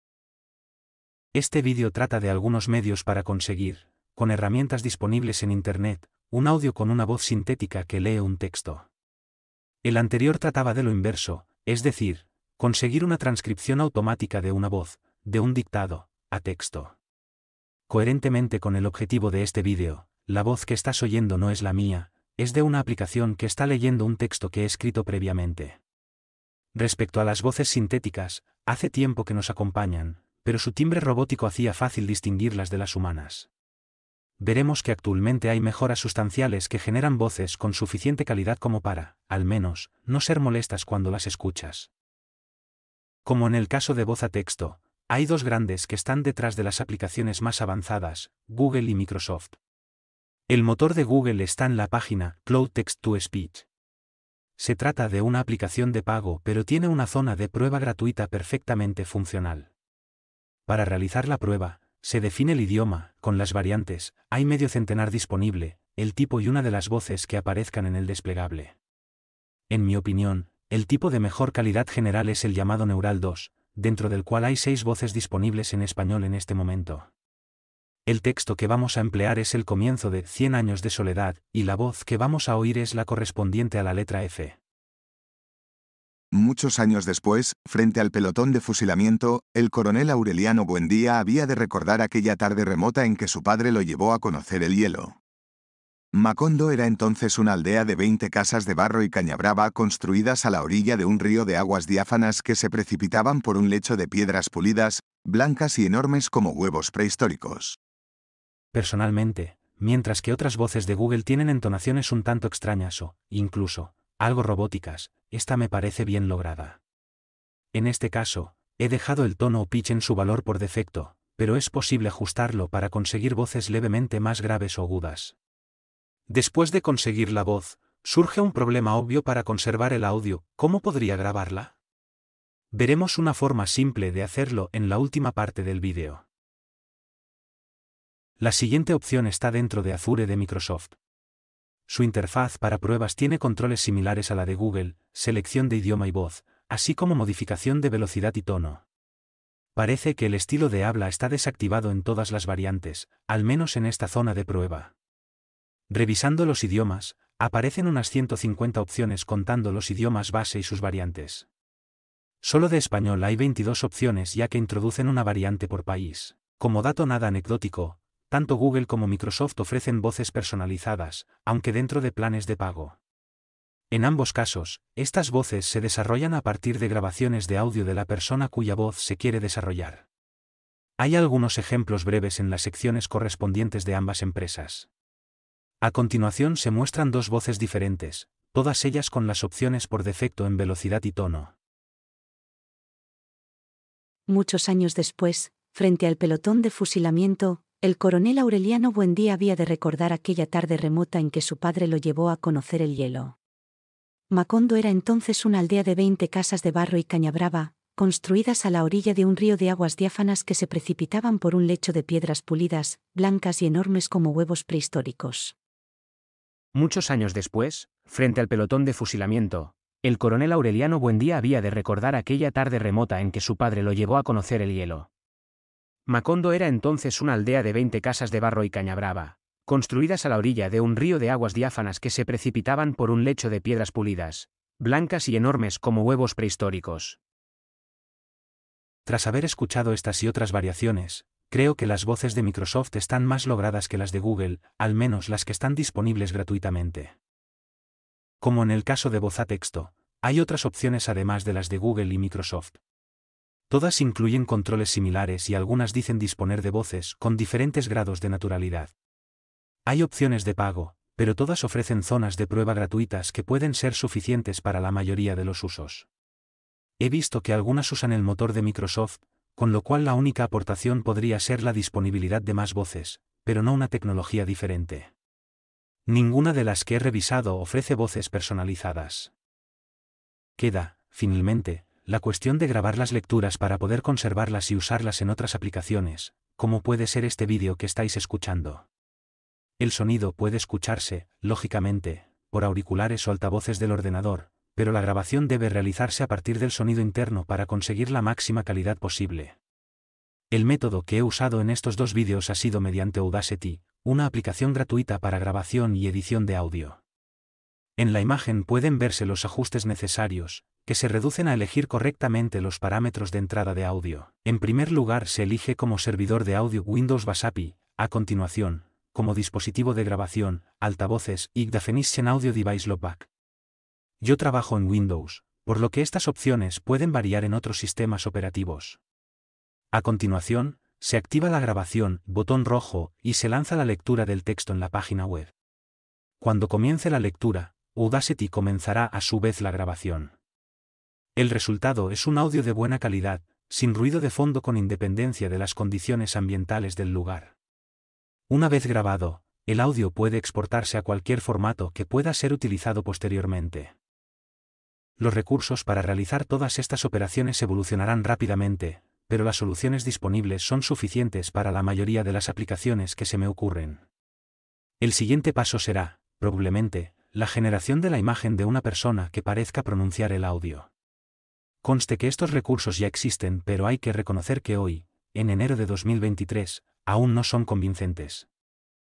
De texto a audio. Cómo hacer que un texto sea leído por una voz sintética razonablemente humana.
Este video trata de algunos medios para conseguir, con herramientas disponibles en internet, un audio con una voz sintética que lee un texto. El anterior trataba de lo inverso, es decir, conseguir una transcripción automática de una voz, de un dictado, a texto. Respecto a las voces sintéticas, hace tiempo que nos acompañan, pero su timbre robótico hacía fácil distinguirlas de las realmente humanas. Veremos que actualmente hay mejoras sustanciales que generan voces con suficiente calidad como para, al menos, no ser molestas cuando las escuchas.